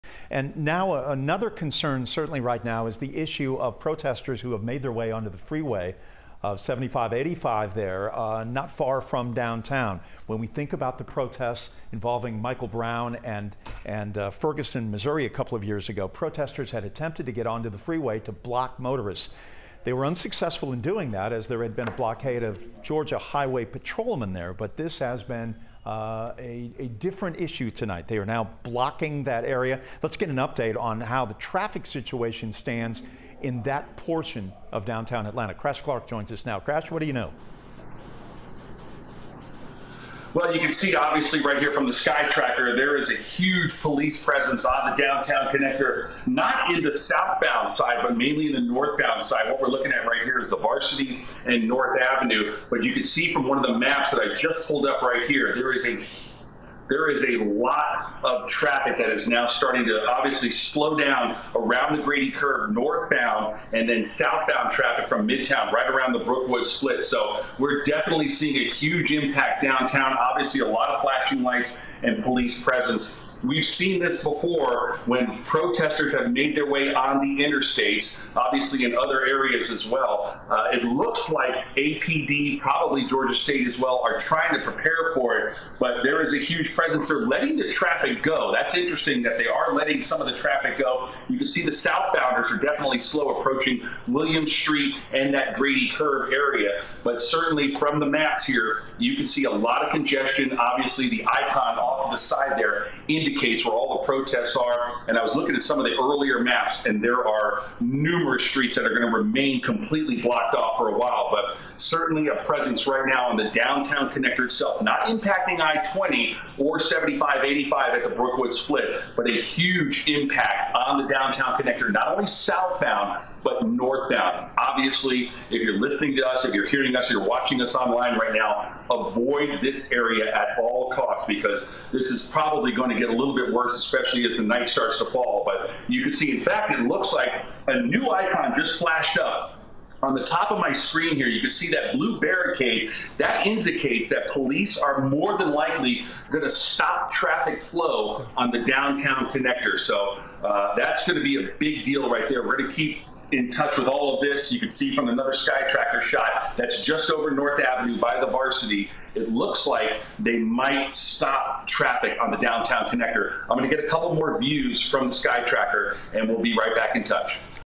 Huge police presence on Downtown Connector in Atlanta after protesters enter roadway
Drivers should avoid the Downtown Connector as protests there, and elsewhere, continue.